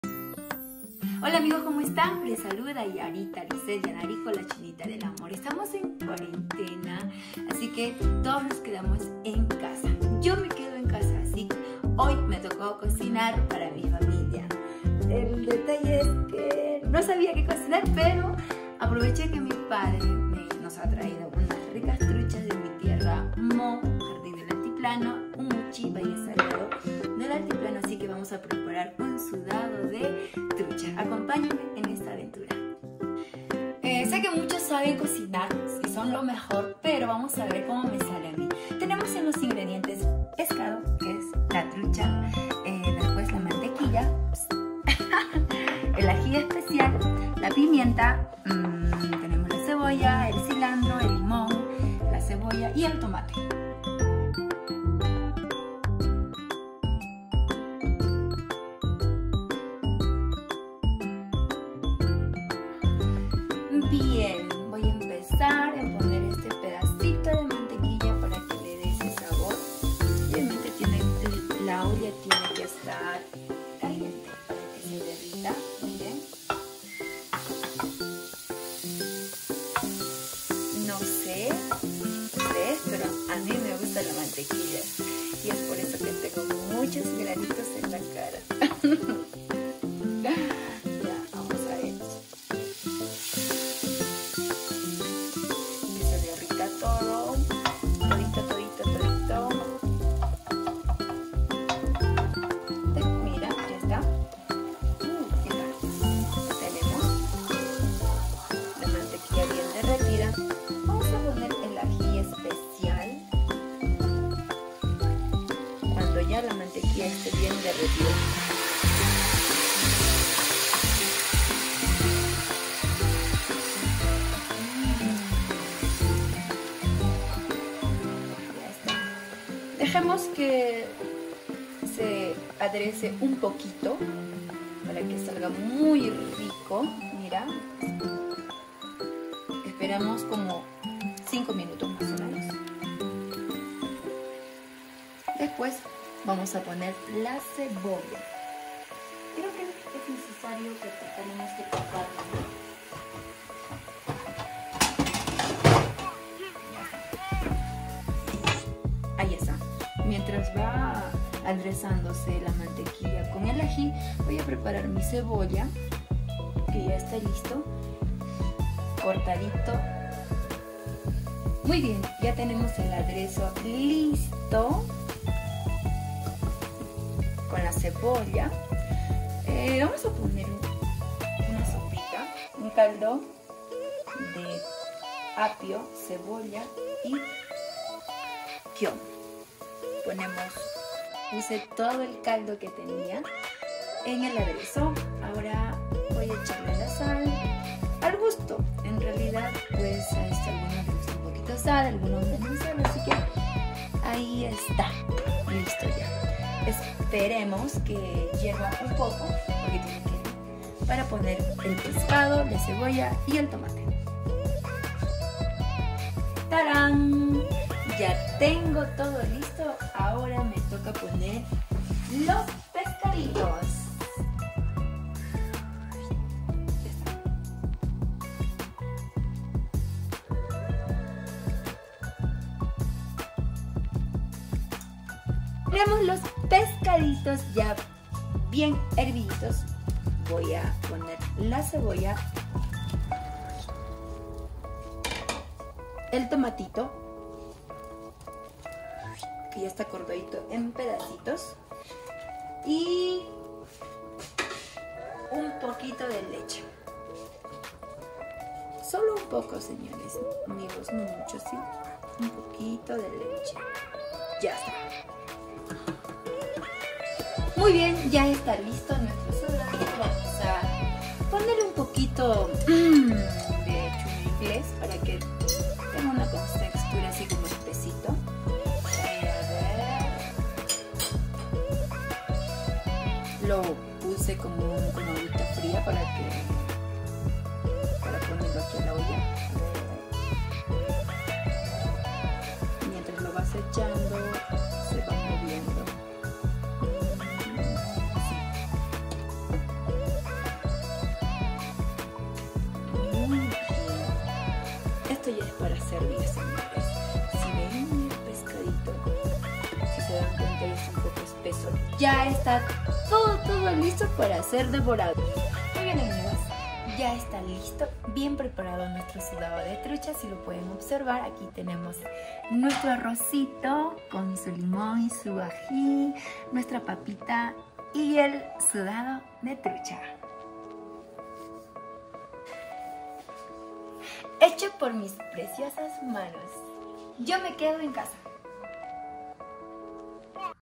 Cocinando con Yarita Lizeth Yanarico - Sudado de Trucha
Comparte conmigo tu receta, hoy me toco cocinar en casita, espero les guste mi video 😇😇